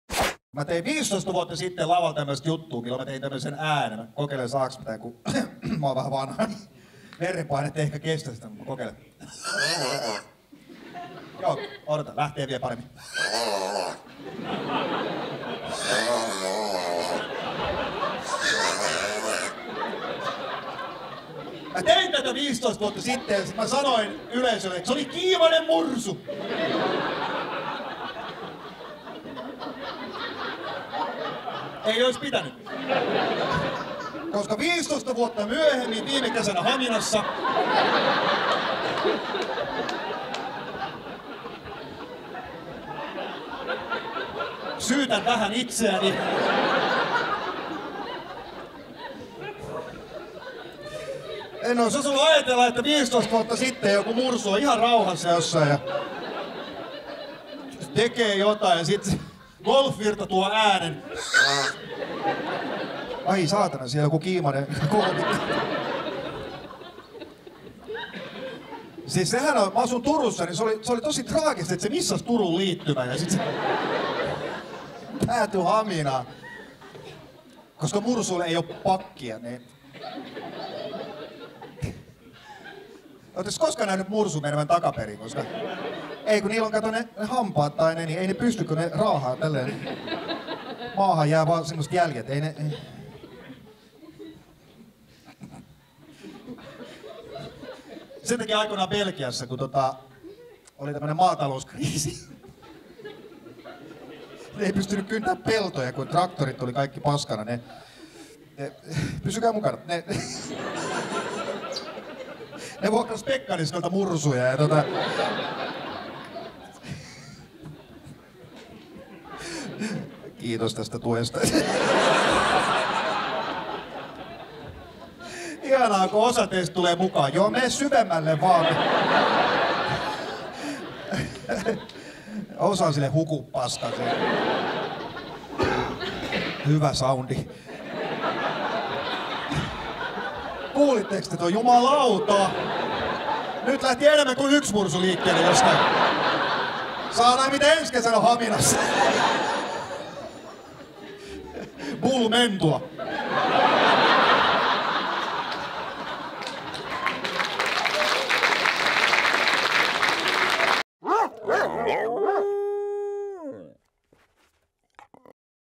Mika Eirtovaara | Stand Up Suomi | Haminan Mursu
Stand up -koomikko Mika Eirtovaaran keikkataltionti Lappeenrannan Sikahauska -festivaaleilta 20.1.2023. Miten Haminan mursu päätyi tulemaan Suomeen?

Keikka meinaa lähteä käsistä, mutta onneksi yleisö kannatteli sitä loppuun asti.

STAND UP!